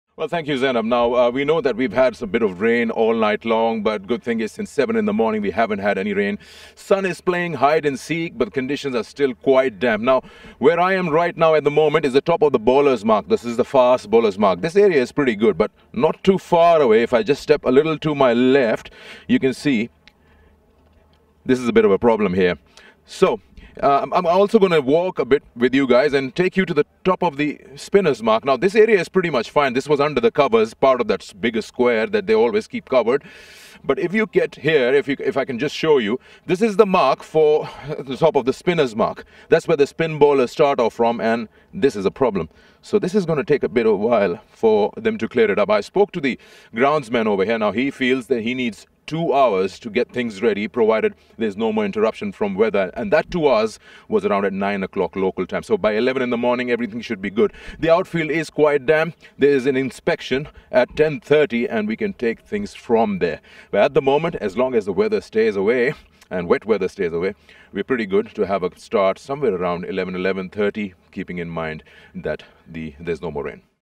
Let's hear from Sikandar Bakht on how the weather is expected to be today | PCB | MM2T
Let's hear from Sikandar Bakht on how the weather is expected to be today.